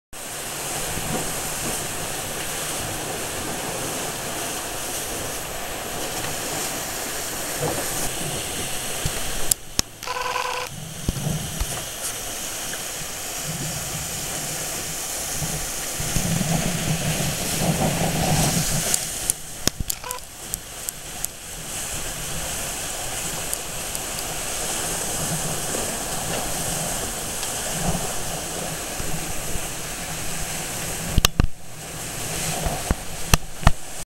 RO Delivery.wmv
Delivering Repeat Offender - just a short vid over the top of the Coromandel